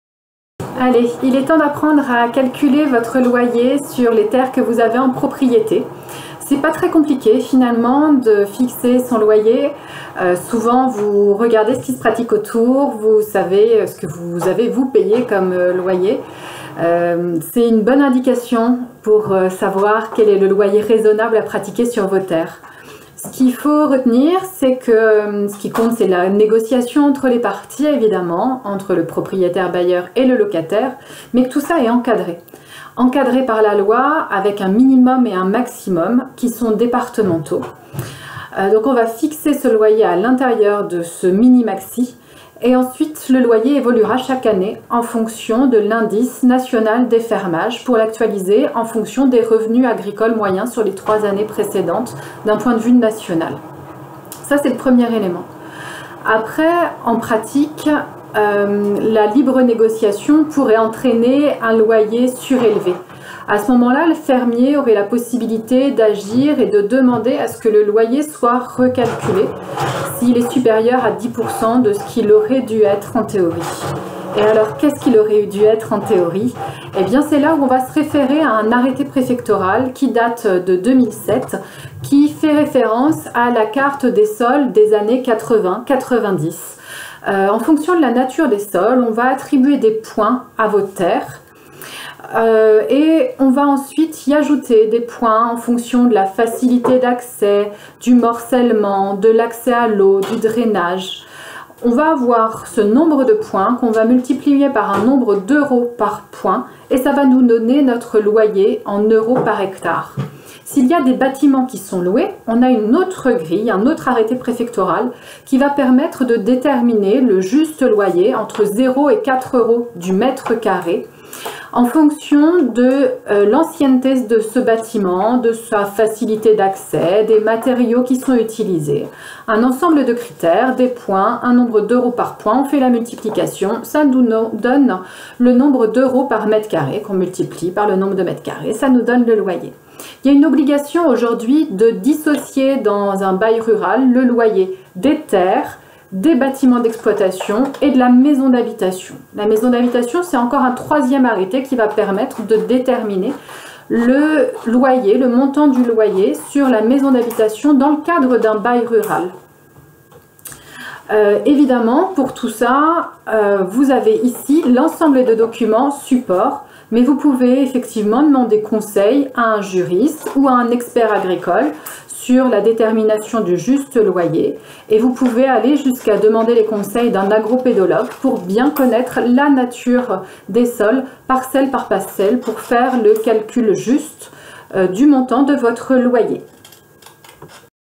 Statut du fermage : comment fixer le loyer de mon bail rural
Statut du fermage, Baux ruraux
Module de formation à distance  pour la formation mixte digitale : "Cap Transmission : Quelle stratégie ?" Chambre d'agriculture Centre - Val de Loire
par Aurélie BRUNET, conseiller transmission à la Chambre d'agriculture de l'Indre